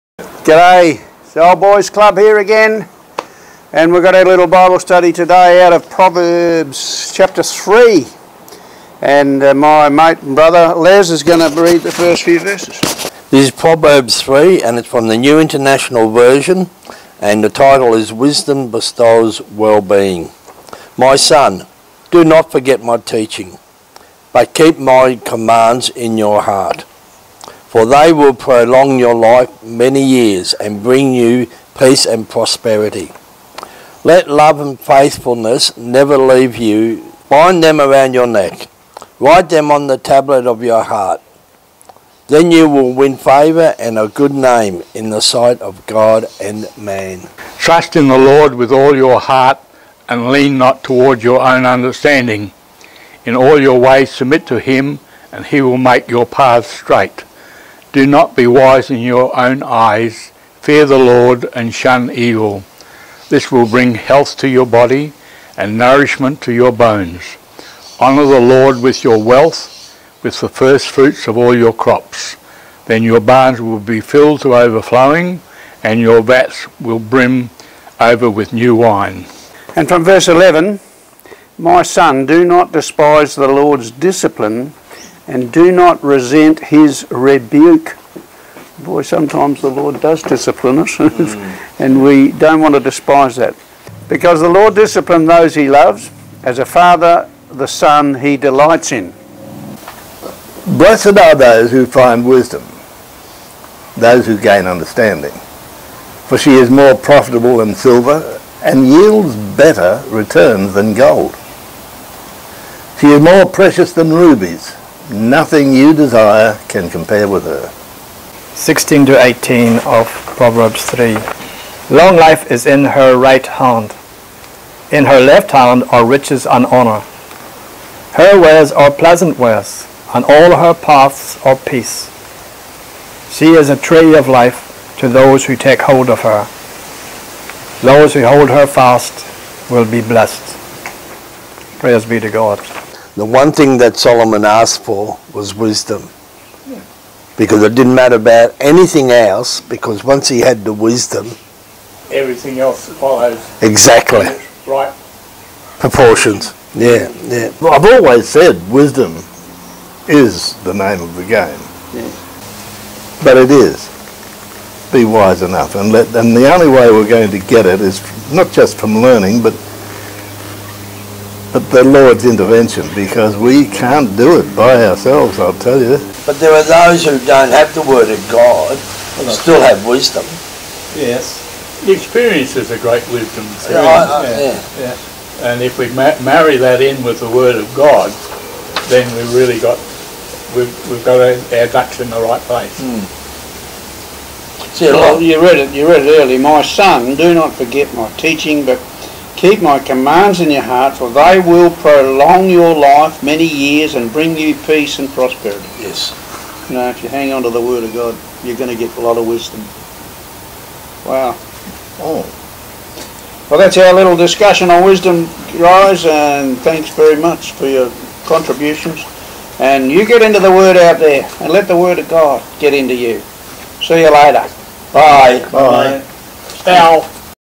How To Find Long Life, Riches and Honor
The Old Boys Club  in Coolum Beach Qld share some wisdom
Aussie Bible Study - Proverbs 3 New International Version (NIV)
Wisdom Bestows Well-Being
1 My son, do not forget my teaching,
    but keep my commands in your heart,
2 for they will prolong your life many years
    and bring you peace and prosperity.
3 Let love and faithfulness never leave you;
    bind them around your neck,
    write them on the tablet of your heart.
4 Then you will win favor and a good name
    in the sight of God and man.
5 Trust in the LORD with all your heart
    and lean not on your own understanding;
6 in all your ways submit to him,
    and he will make your paths straight.
7 Do not be wise in your own eyes;
    fear the LORD and shun evil.
8 This will bring health to your body
    and nourishment to your bones.
9 Honor the LORD with your wealth,
    with the first fruits of all your crops;
10 then your barns will be filled to overflowing,
    and your vats will brim over with new wine.
11 My son, do not despise the LORD’s discipline,
    and do not resent his rebuke,
12 because the LORD disciplines those he loves,
    as a father the son he delights in.
13 Blessed are those who find wisdom,
    those who gain understanding,
14 for she is more profitable than silver
    and yields better returns than gold.
15 She is more precious than rubies;
    nothing you desire can compare with her.
16 Long life is in her right hand;
    in her left hand are riches and honor.
17 Her ways are pleasant ways,
    and all her paths are peace.
18 She is a tree of life to those who take hold of her;
    those who hold her fast will be blessed.